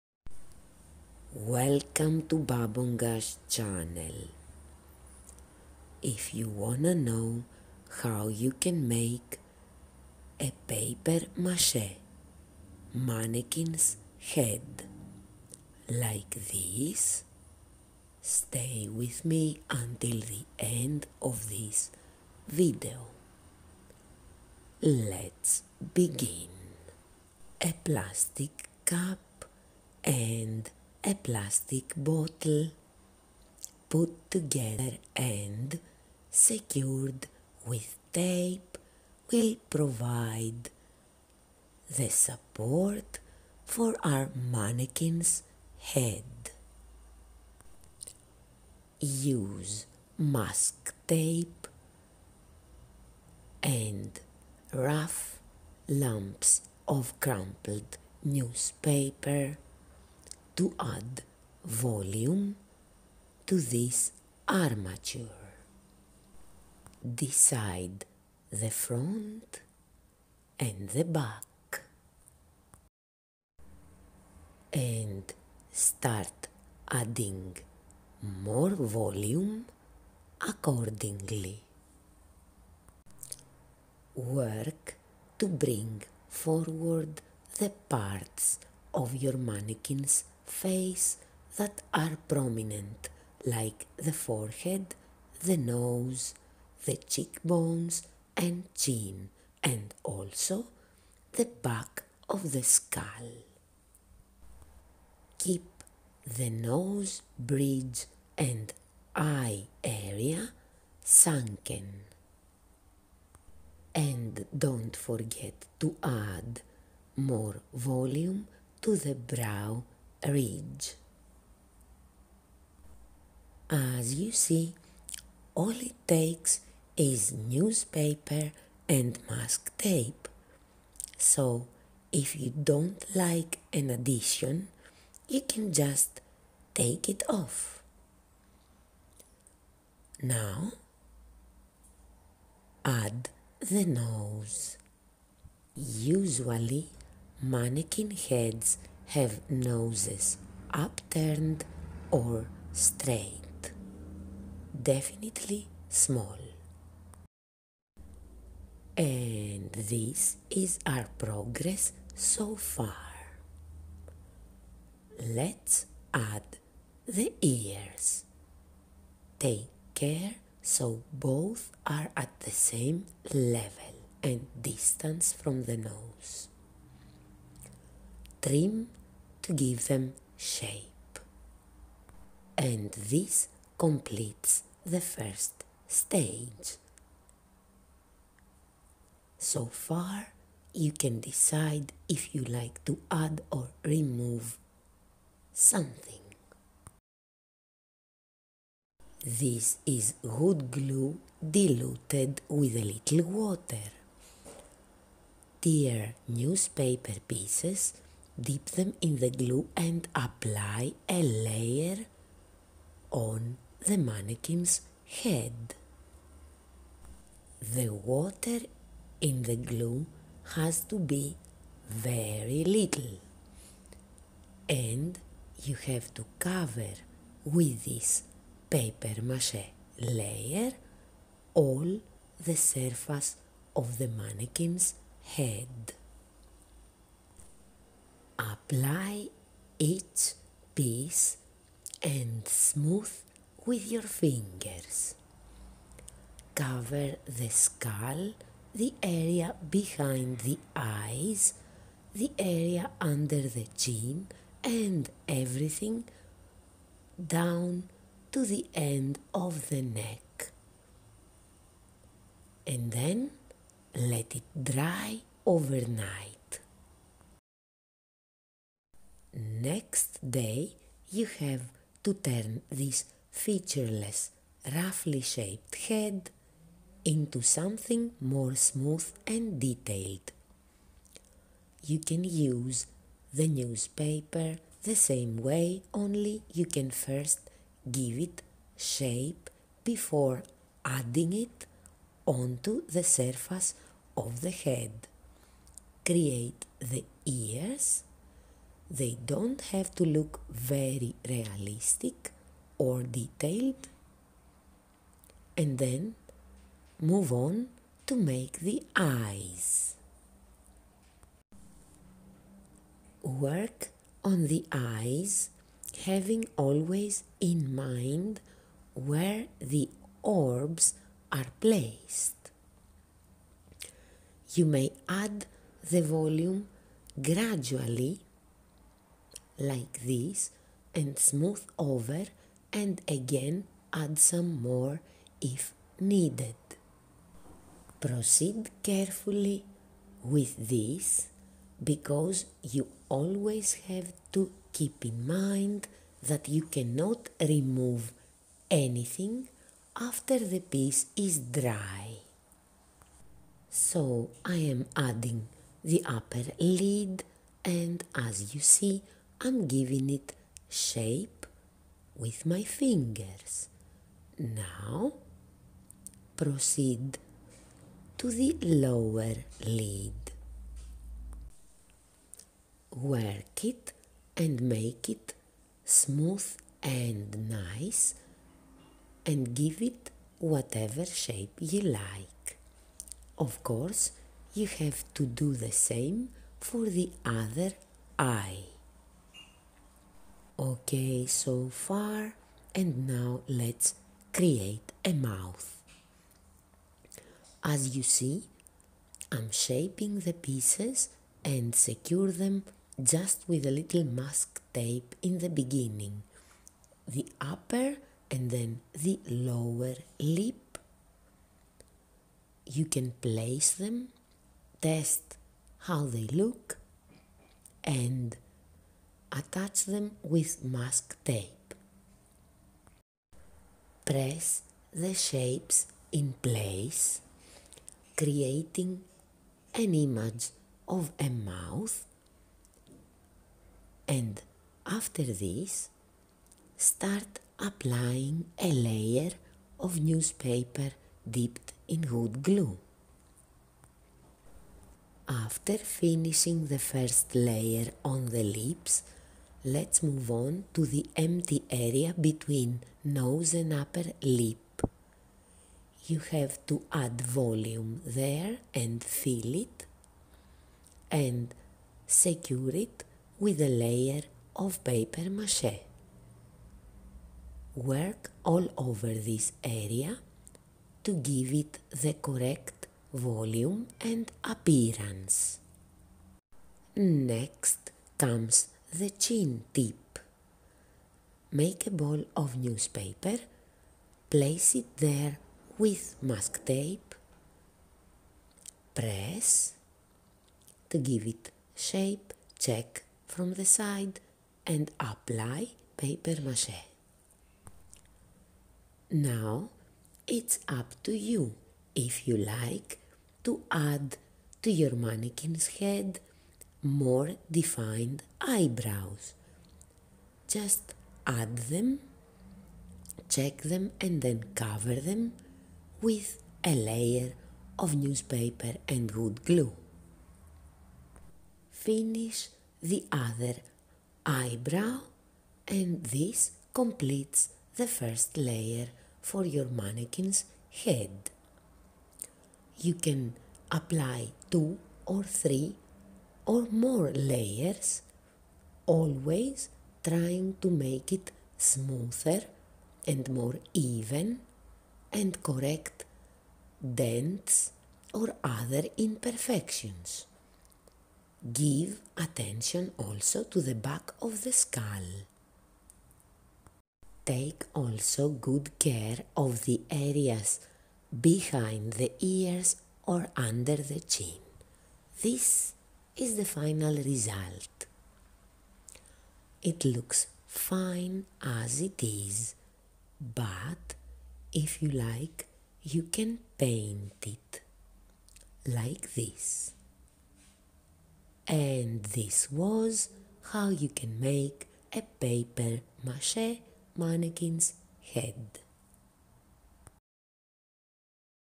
Mannequin head! Paper mache tutorial
Make an easy mannequin head from simple paper mache (no clay or pulp) with this quick tutorial. You only need a plastic bottle and cup (for support), old newspapers, mask tape and wood glue. If you like to paint it, plastic colors work fine on this kind of paper mache.